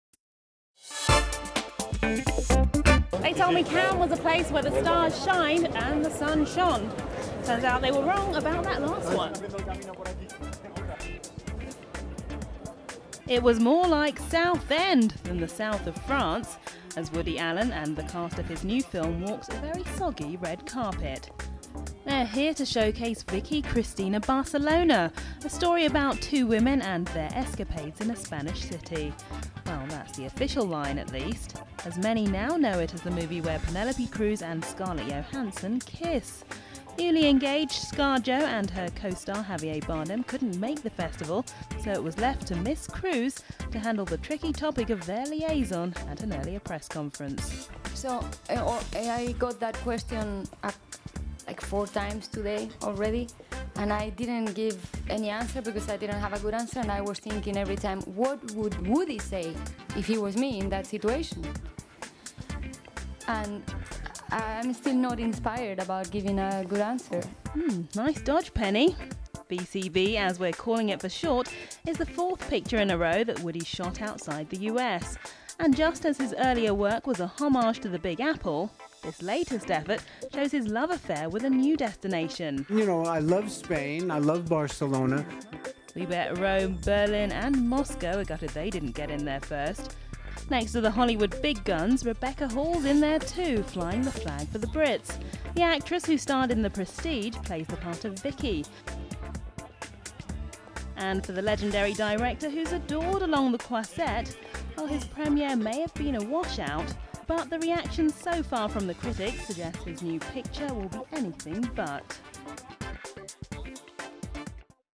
Woody Allen debuts in Cannes
One of the Cannes film festival's best loved directors, Woody Allen, has returned with his new film, Vicky Cristina Barcelona. Test